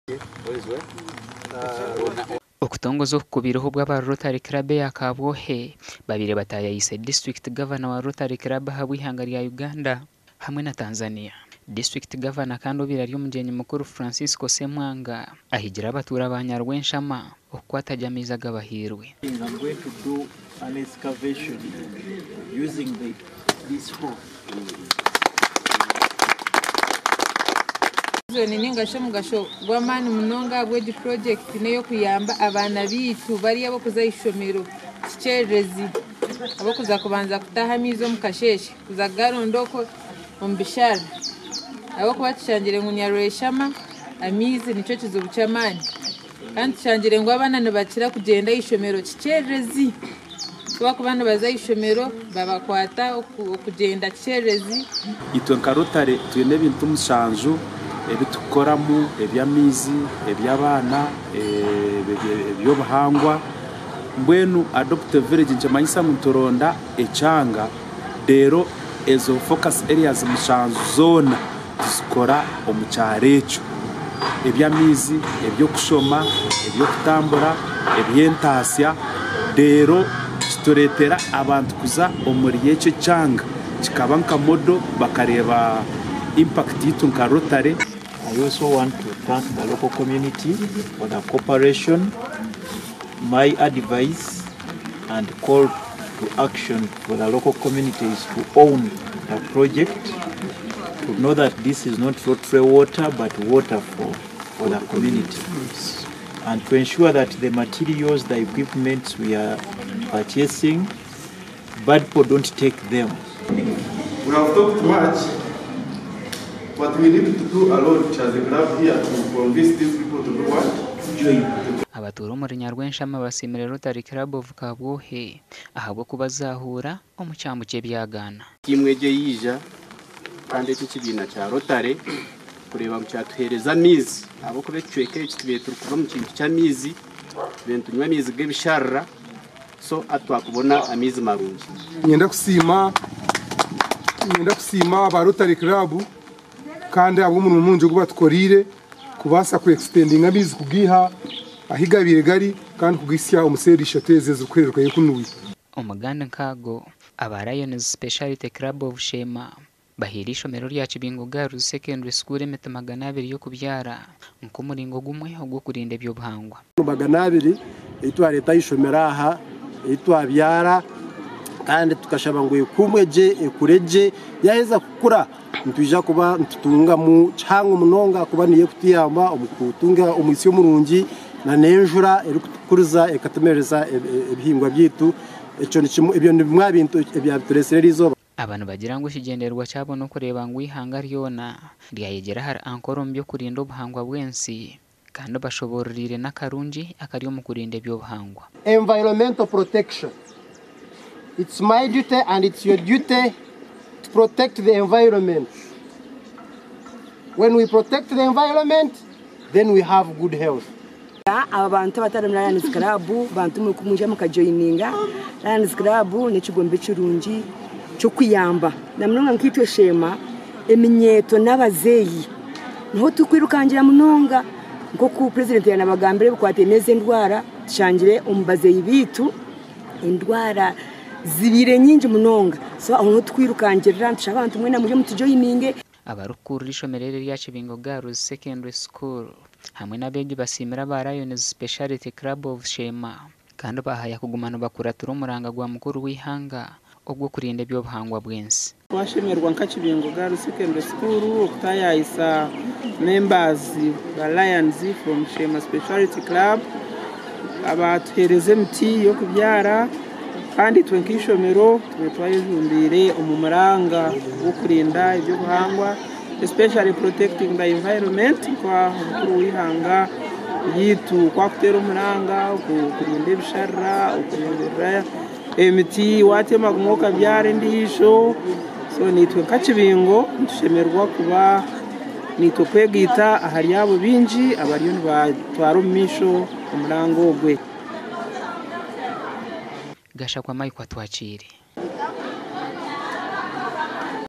AGEIHANGWE: Aba Rotary Club Kabwohe bahiire ebyaro 4 amaizi- Sheema.
SHEEMA:

Aba Rotary Club Kabwohe bahiire ebyaro 4 amaizi...